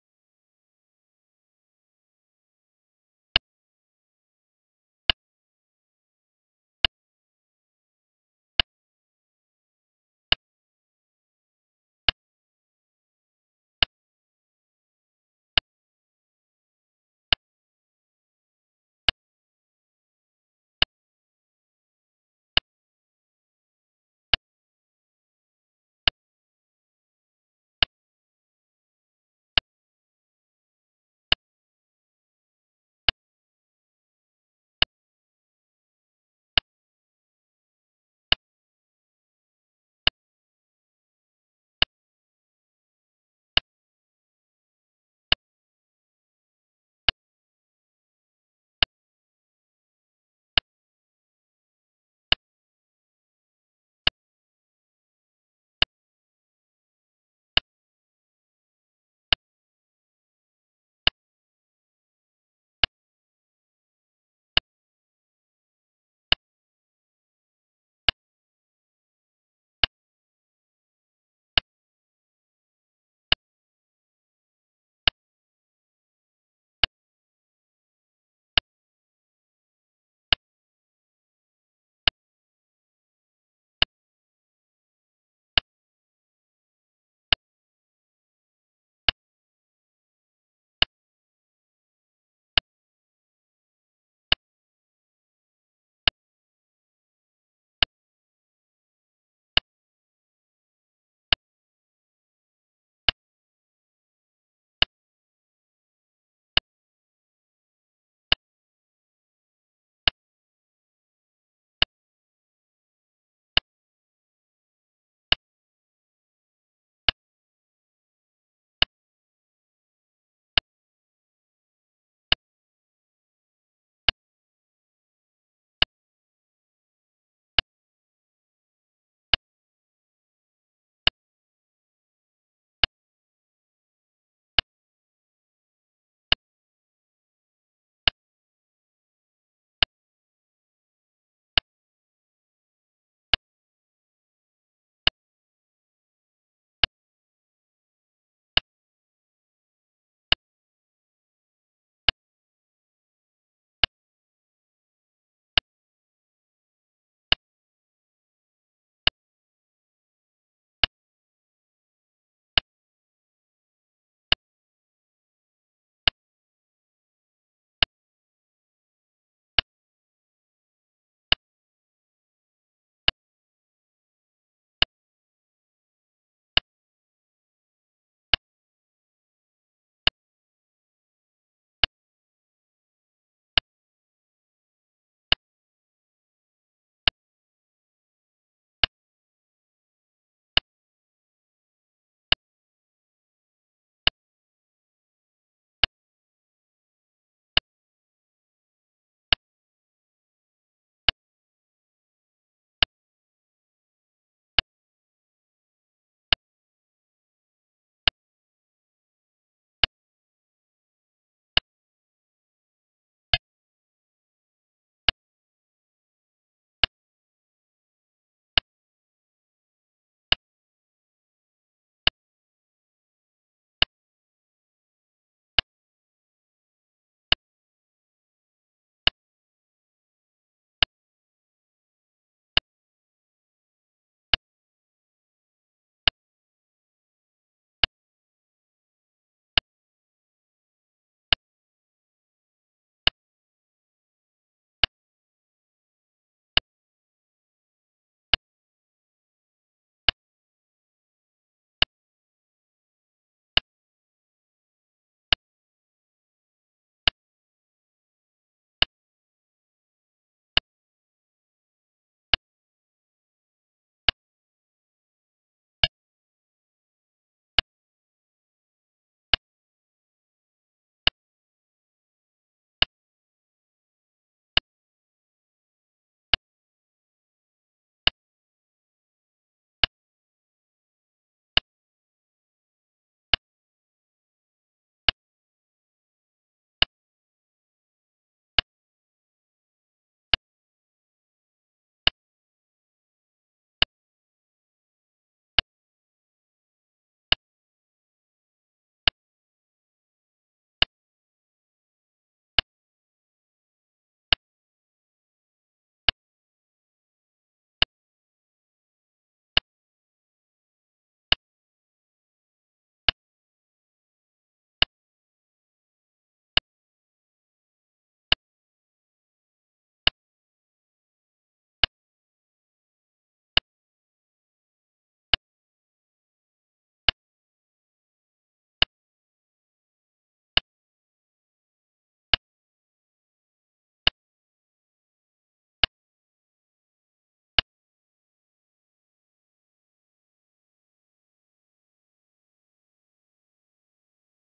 2019「福建海峡銀行杯」7回戦 LeelaZero（黒）vs 超然围棋（白）
2019「福建海峡銀行杯」7回戦（全対局）

小爱围棋（黒）vs 飞虎队（白）黒不戦勝